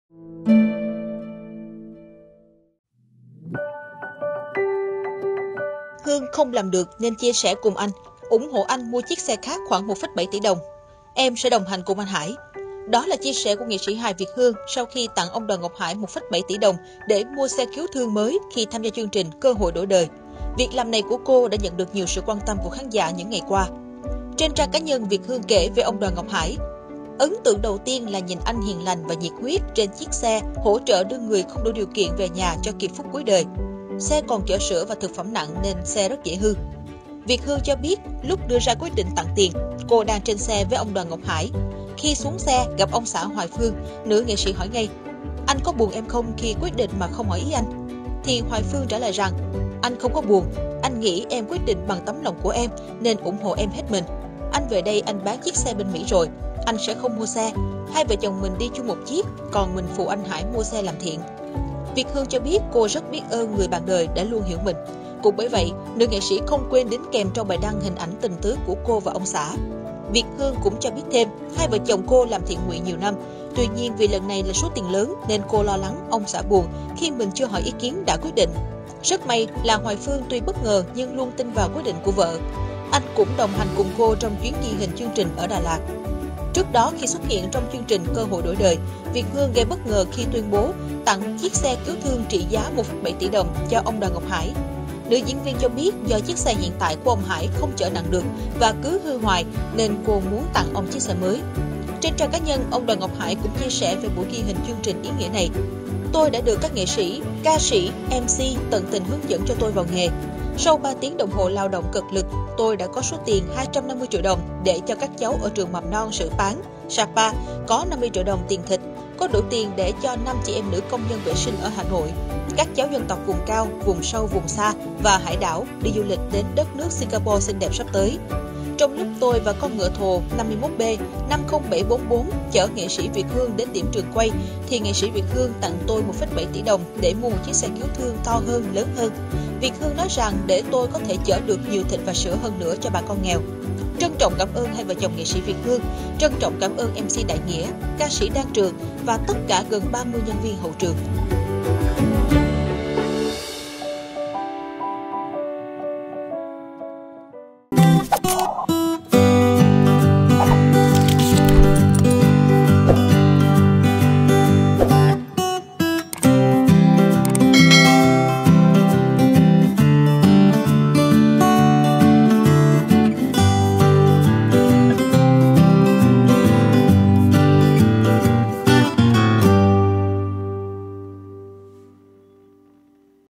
Tặng ông Đoàn Ngọc Hải 1,7 tỉ mua xe cấp cứu, Việt Hương được chồng nói 1 câu ai cũng ấm lòng
Nghệ sĩ Việt Hương có những chia sẻ gây chú ý về việc tặng ông Đoàn Ngọc Hải 1,7 tỉ đồng mua xe cứu thương mới đang được dư luận quan tâm những ngày gần đây.